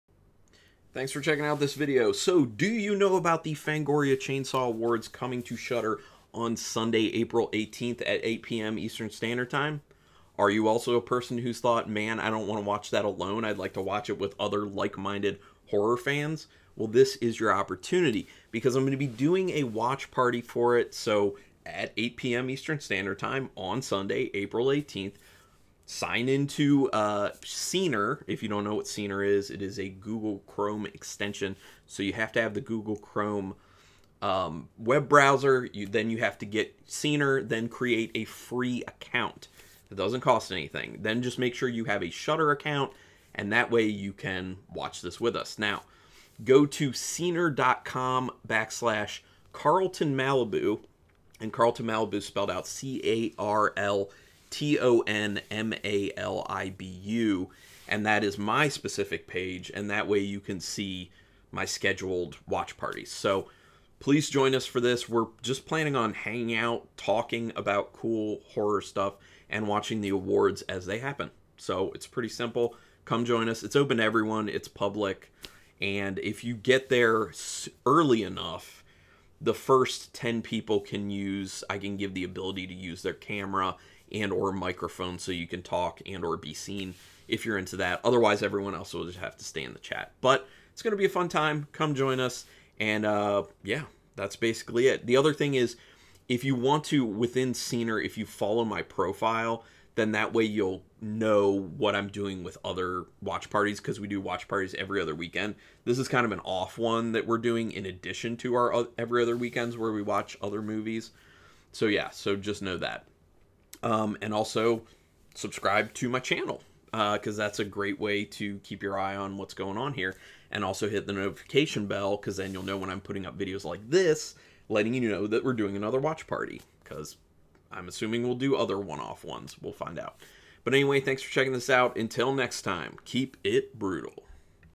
Fangoria Chainsaw Awards Watch Party!
Carlin gives the details on a watch party he's hosting for the 2021 Fangoria Chainsaw Awards on Shudder.

Scener.com/CarltonMalibu